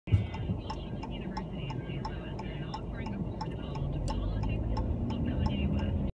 Fairmount Ave East (take 433): Yaris compact running the red light
Yaris compact running the red light please check out my twitter feed @thcsoy